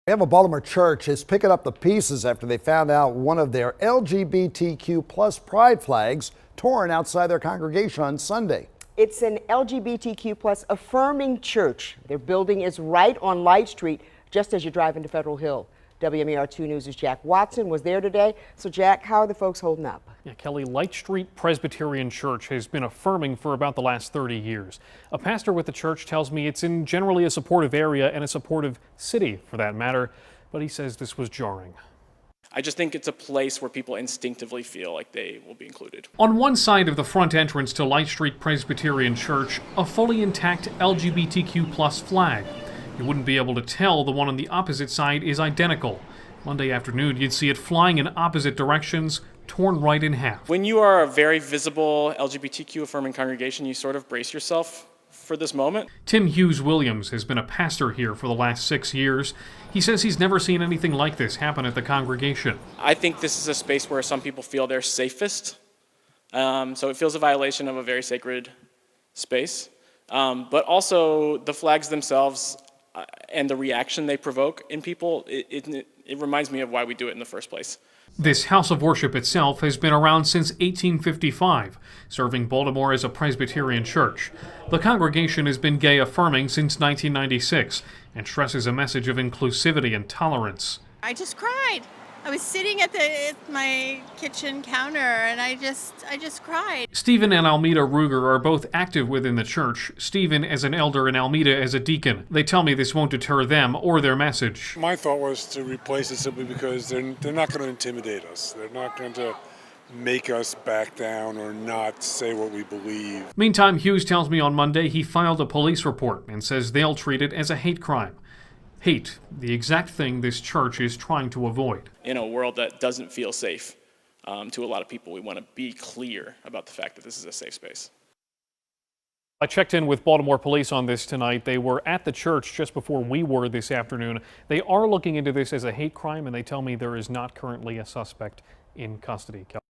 Pride flag torn outside Federal Hill church
A Baltimore church found one of its LGBTQ+ Pride flags torn outside their congregation on Sunday.

Light Street Presbyterian Church in Federal Hill is an LGBTQ-affirming church and has been since 1996.

Baltimore Police confirmed to WMAR-2 News that they are treating the incident as a hate crime.

Tim Hughes Williams, a pastor with the church, says the church is in a generally supportive area and a supportive city, but this was upsetting.

"When you are a very visible LGBTQ-affirming congregation, you sort of brace yourself for this moment," Hughes said.